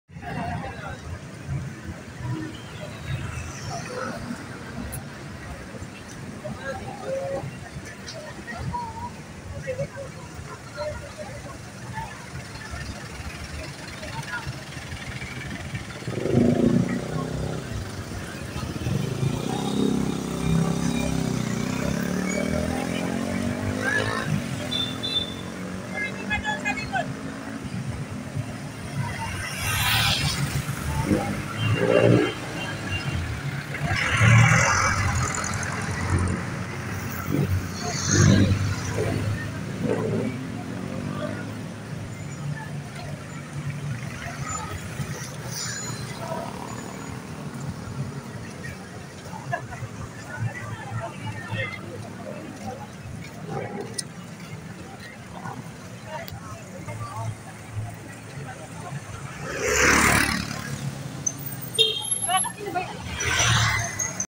Mahabang pila tuwiNg Gabi,,Ng mga papauwing pasahero papuntang Novaliches
Malinta Deritso terminal ,,